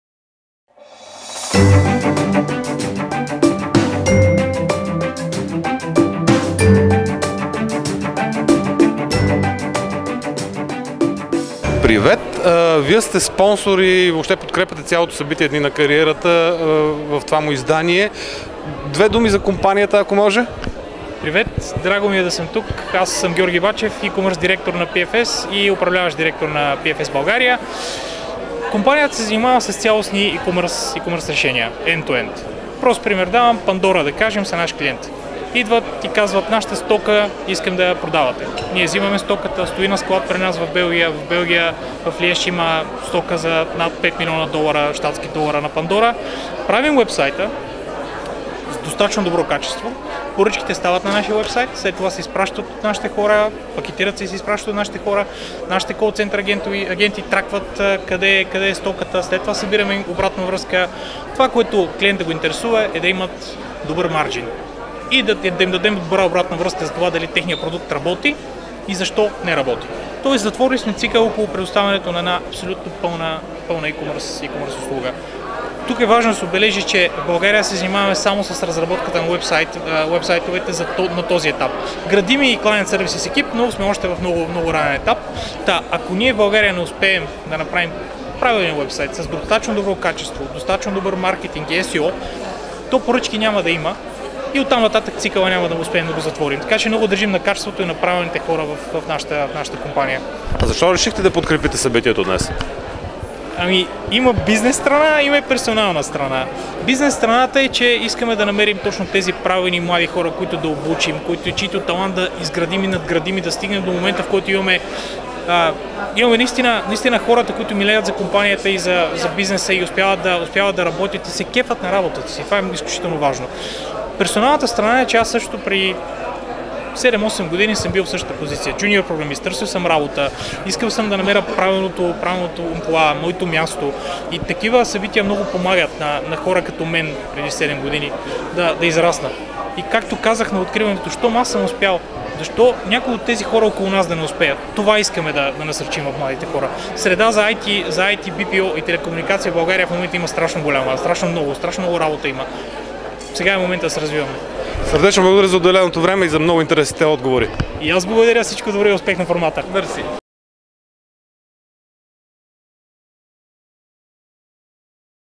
Георги Бачев, PFS: "След като аз съм успял, защо и другите да не успеят също?"
Премина успешно 7-то поредно издание на единствения в България кариерен форум, насочен към компании в секторите: ИТ, Телекомуникации и Business Process Outsourcing (ВРО). То се състоя на 13 октомври в София и за пръв път, на 14 октомври в Пловдив и беше посетено от над 1000 млади хора, мнозинството от които бяха студенти, активно търсещи работа.

В това издание на Дни на кариерата участваха и рекорден брой компании – 79, които представяха своята дейност, както и възможностите, които предоставят за работа и развитие на кариера в България.

PFS е компания, която се занимава с цялостни e-commerce решения. Това означава, че фирмата предлага end-to-end услуги. Както сподели пред нашата камера Георги Бачев, E-commerce директор на PFS и Изпълнителен директор на PFS България, това означава абсолютно затворен цикъл. Компанията клиент оставя стоката си на склад в PFS, PFS пък създава сайта за търговия на клиента, поема поръчките, осигурява доставката до крайния клиент и дори събира обратна връзка. Така клиентът се е погрижил за целия процес, благодарение на PFS. На този етап, българското поделение се занимава само с изработката на уебсайтовете.